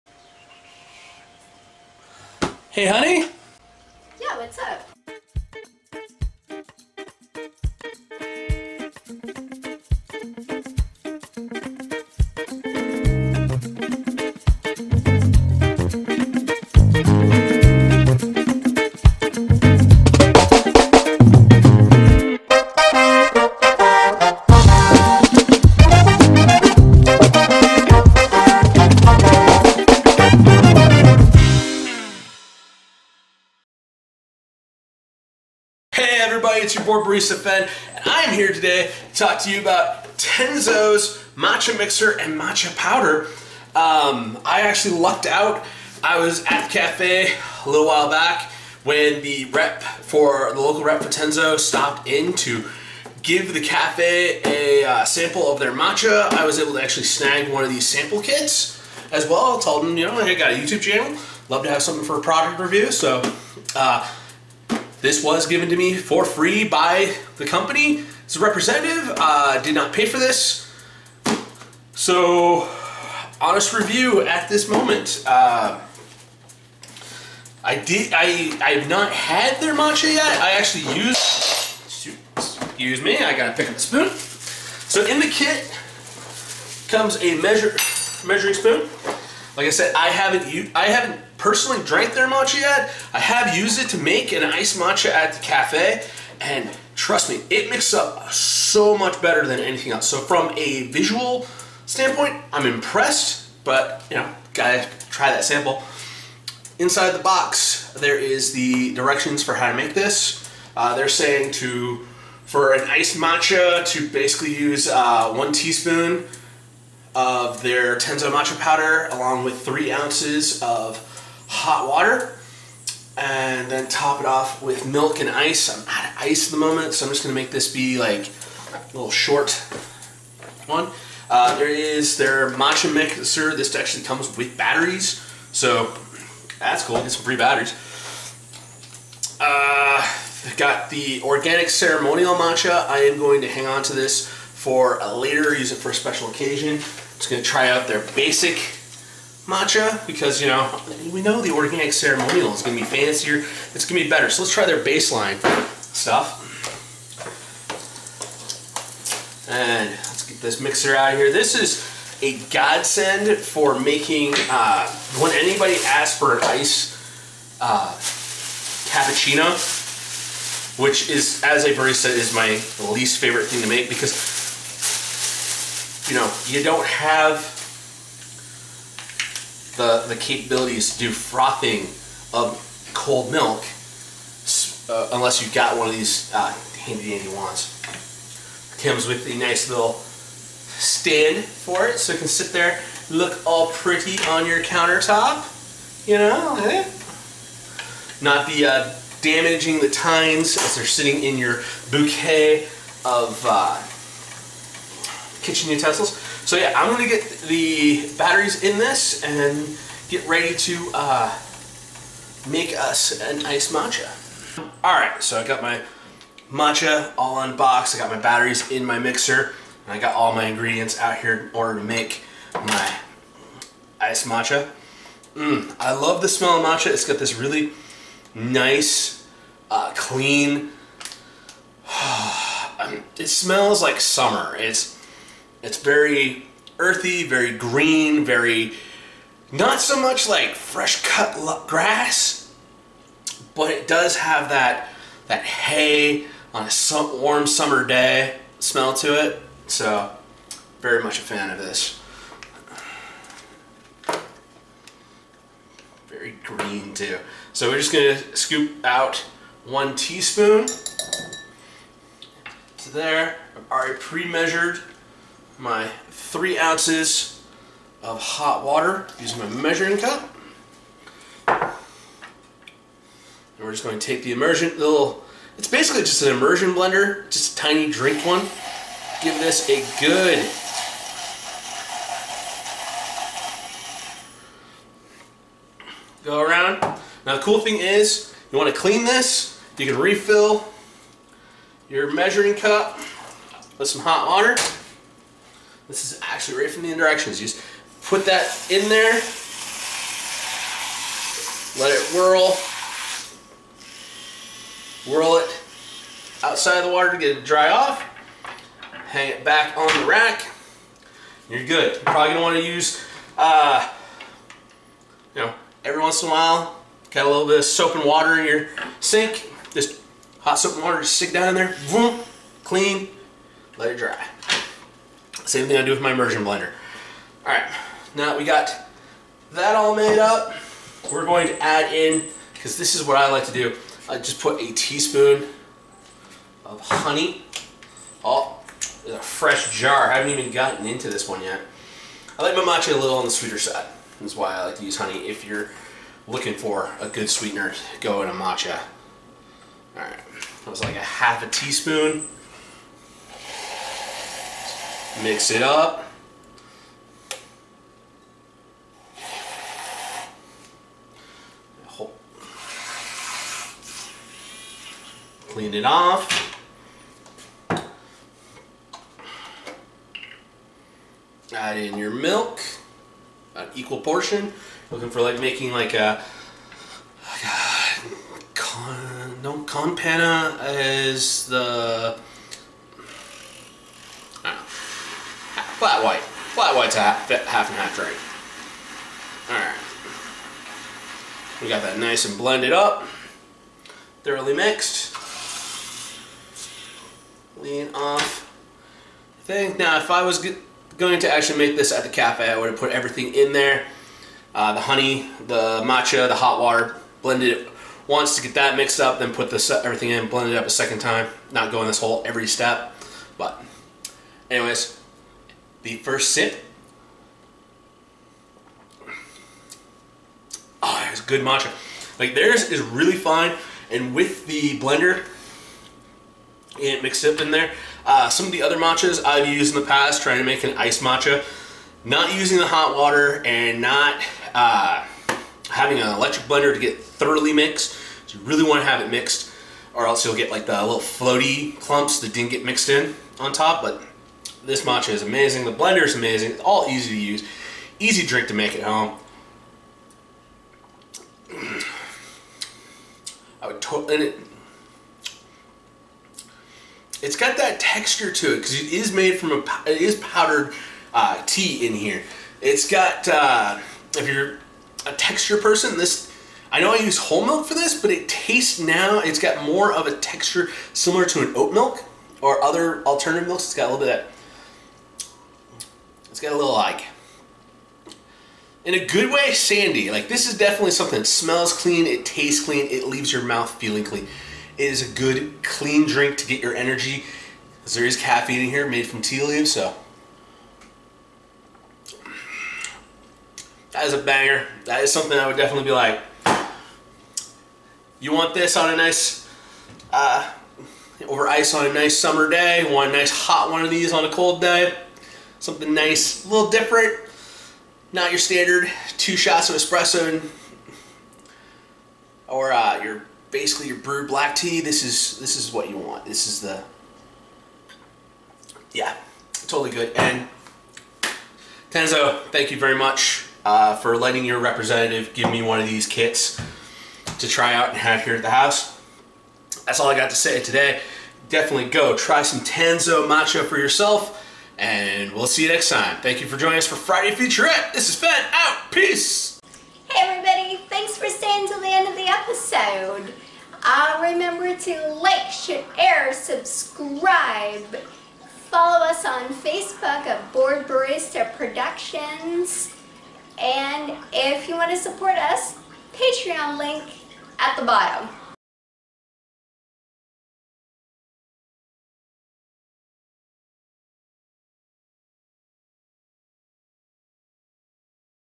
Friday Featurette - Tenzo Matcha Kit Review
So Fen was given a Tenzo matcha sample kit to try and review. So we are making Fen's recipe for a honey matcha. We didn't pay for this kit and were given it freely by a local rep with no monetary compensation or expectations to make this video. This is an unbiased review. Enjoy.

Support Fen’s hoarding of plastic soldiers and other bad habits